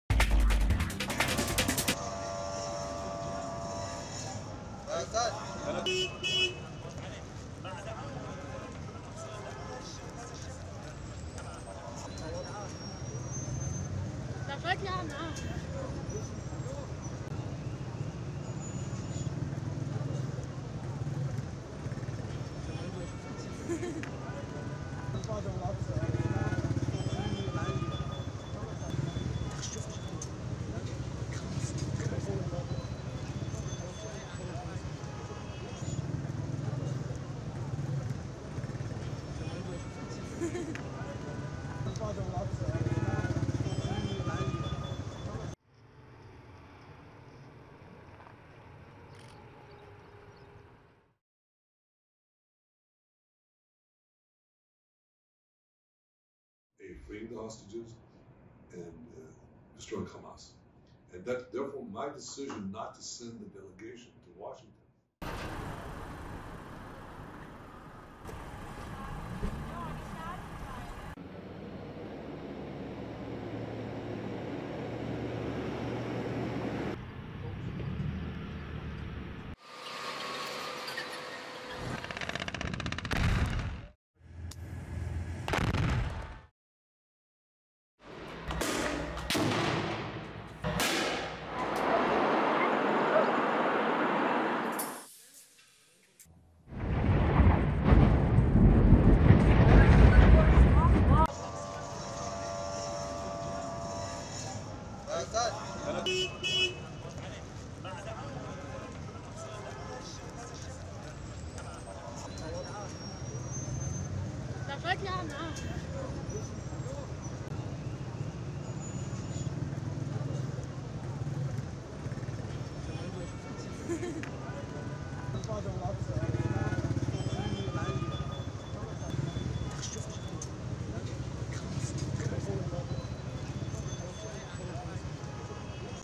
Israel vs Iran | Iran Vows Revenge After Top Commander Is Killed In An Israeli Airstrike | N18V
A suspected Israeli strike has destroyed the Iranian consulate building in Syria's capital, Damascus and killed at least seven people. A senior Revolutionary Guards commander Brig-Gen Mohammad Reza Zahedi is among the dead. This is an alarming escalation of the conflict in the Middle East that would pit Israel against Iran and its allies. Iran has vowed a ‘decisive response’ to the killing of its top commander. Since the Gaza war began, Israel has ramped up airstrikes in Syria against Lebanon's Iranian-backed Hezbollah militia and Iran's Guards.

n18oc_news